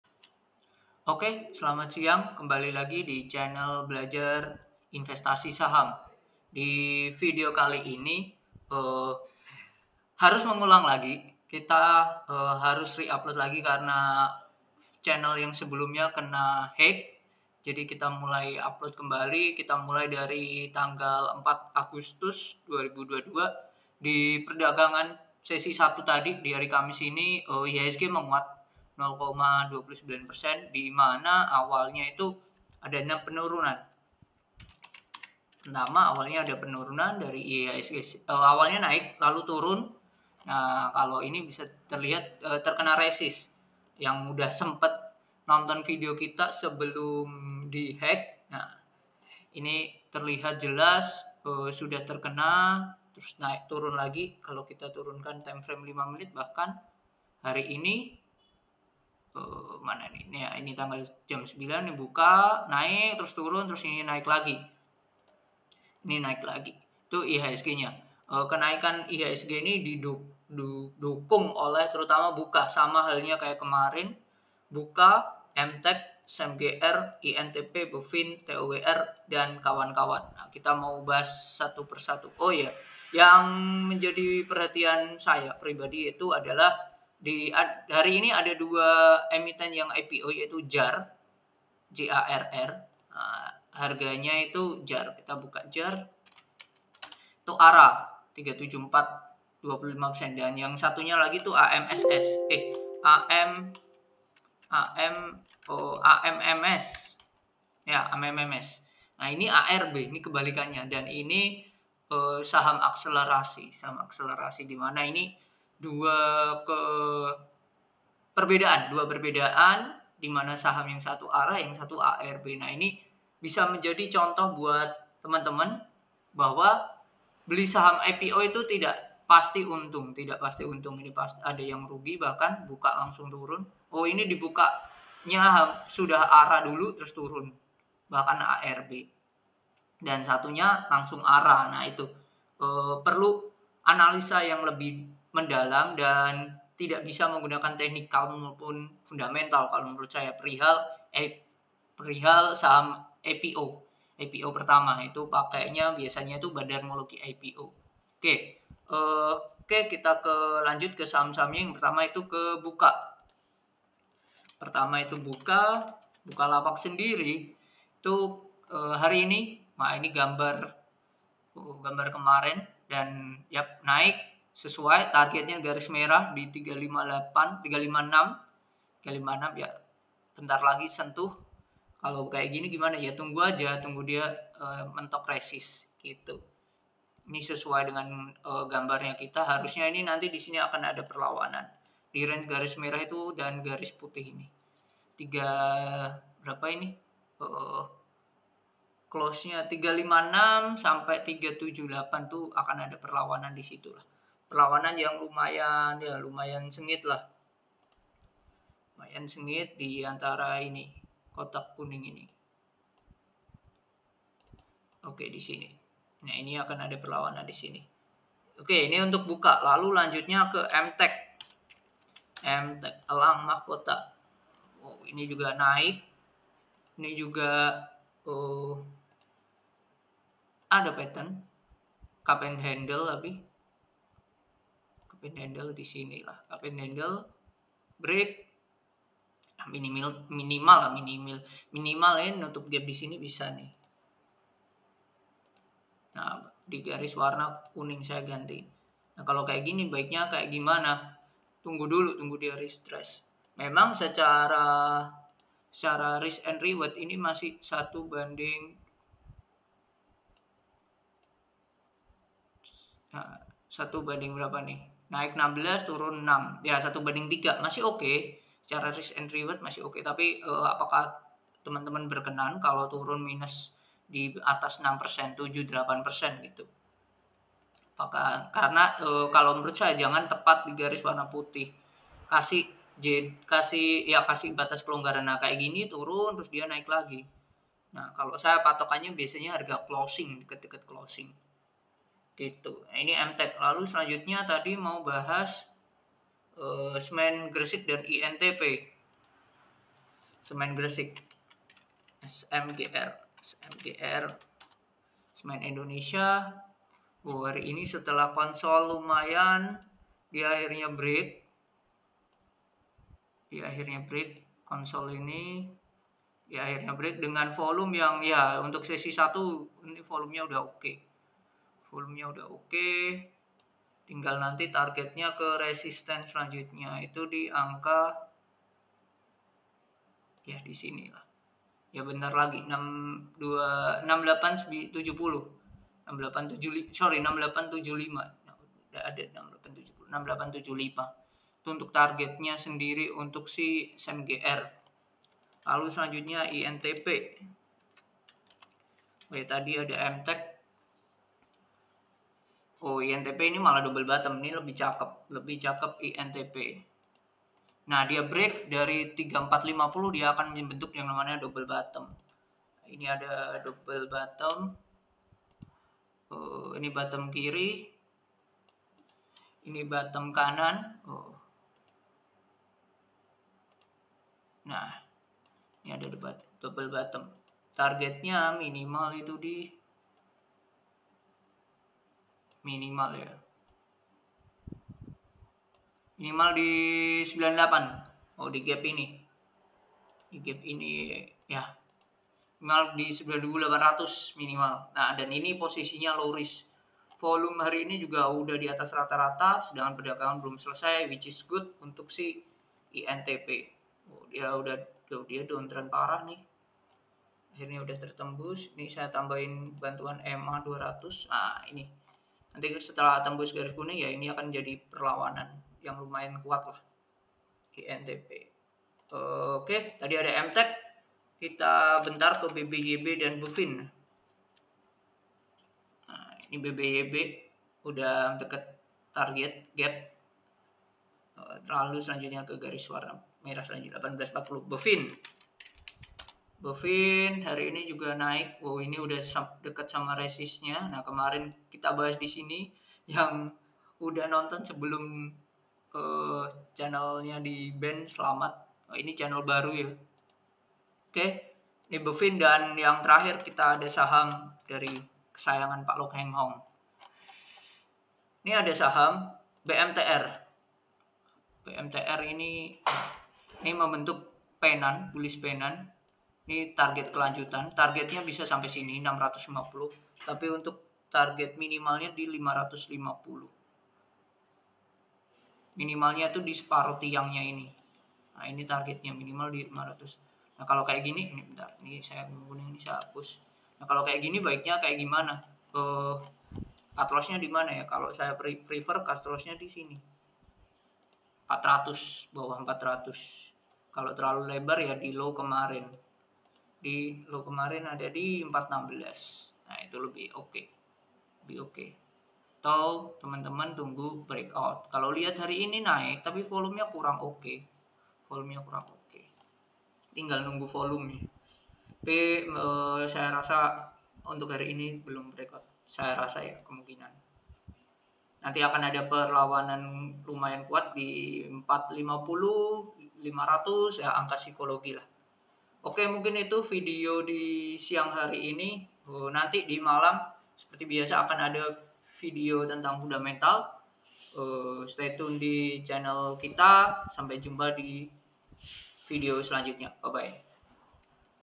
ANALISA TEKNIKAL! SAHAM KESAYANGAN PAK LKH NIH, BMTR GOES TO 550 / LEMBAR.
Saham kesayangan pak LKH nih. BMTR bmtr menuju 550 / lembar.
Selain BMTR, ada analisa teknikal saham-saham yang naik kencang di perdagangan sesi 1 pada 4 Agustus 2022. emtk buka smgr intp bmtr bbyb bfin berdasarkan analisa teknikal.

Sebuah channel seputar investasi saham.
Mari Belajar Bersama :)